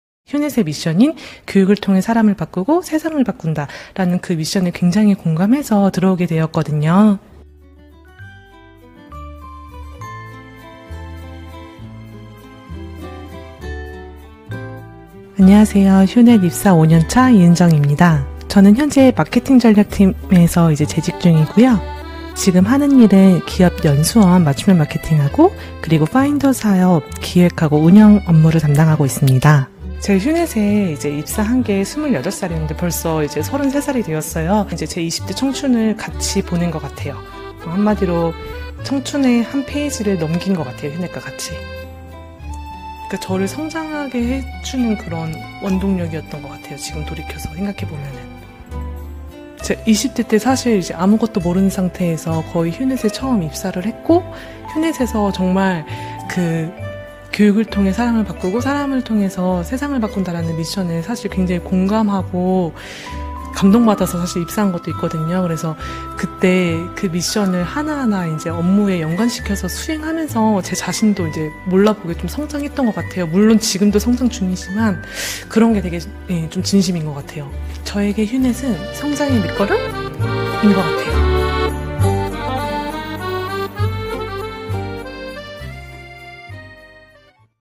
[휴넷 창립 20주년 특집] 장기근속자 인터뷰_입사 5년차, 아련하게 이은정
교육이 사람을 바꾸고
사람이 세상을 바꾼다!

안녕하세요? 휴넷 입사 5년차 이은정입니다.

저는 현재 마케팅전략팀에서 재직중이고
지금 하는 일은 기업 연수원 맞춤형 마케팅과
파인더 사업 기획하고 운영 업무를 담당하고 있습니다.

제가 휴넷에 입사한 게 28살인데 벌써 33살이 되었어요.
제 20대의 청춘을 같이 보낸 것 같아요.
한마디로 청춘의 한 페이지를 넘긴 것 같아요. 휴넷과 같이

저를 성장하게 해주는 원동력이었던 것 같아요.
지금 돌이켜 생각해보면...

20대때 아무것도 모르는 상태에서
휴넷에 처음 입사를 했고

휴넷에서 '교육을 통해 사람을 바꾸고
사람을 통해 세상을 바꾼다'는 미션에 공감하고
감동받아서 입사한 것도 있는데

그 때 그 미션을 하나하나 업무에 연관시켜서 수행하면서
제 자신도 몰라보게 성장했던 것 같아요.
물론 지금도 성장 중이지만 그런 게 진심인 것 같아요.

나에게 휴넷은 성장의 밑거름!

Bravo Your Life!

재밌게 보셨다면 휴넷 구독 버튼 클릭!??